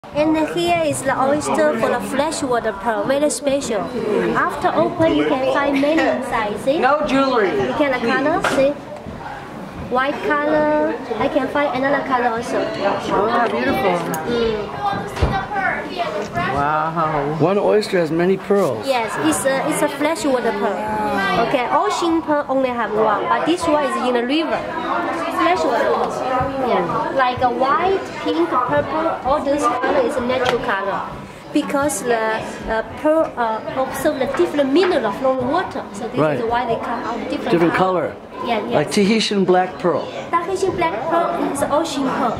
River Pearls of Shanghai
River pearls in Shanghai typically refer to the freshwater pearls cultivated in the rivers around the city. Shanghai, being a city situated along the Yangtze River Delta, has a rich history of pearl cultivation. These pearls are usually cultured in mussels that inhabit the freshwater bodies surrounding the city.
Shanghai river pearls are renowned for their luster, smoothness, and varying colors, which can range from white to cream, pink, lavender, and even slightly golden hues. The pearls are cultivated through a process known as pearl farming, where small irritants or beads are inserted into the mollusk's tissue, encouraging the formation of a pearl around it.